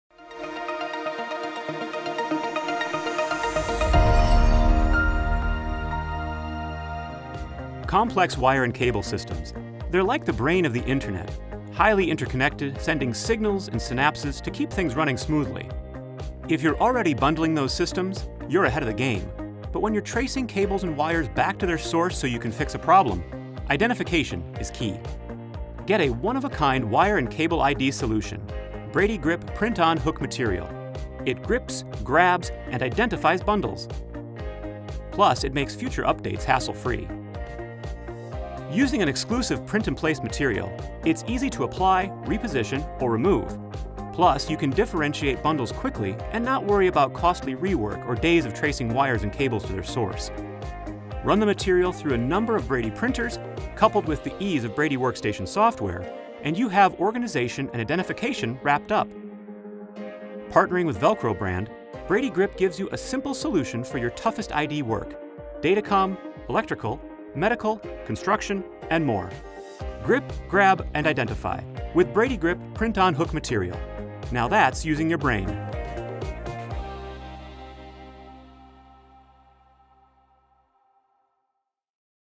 Introducing A New Solution for Cable Management Professionals | VELCRO® Brand & Brady
Fast, reliable cable ID is now at your fingertips. Thanks to Brady and VELCRO® Brand partnership CableManagement just got a whole lot easier. Introducing: BradyGrip™ Print-on Hook Material. A one-of-a-kind solution that allows printed identification to be placed directly on bundles.

A grippy backing makes it easy to reposition or remove, so no need for costly rework. Just print on and place – it’s that simple.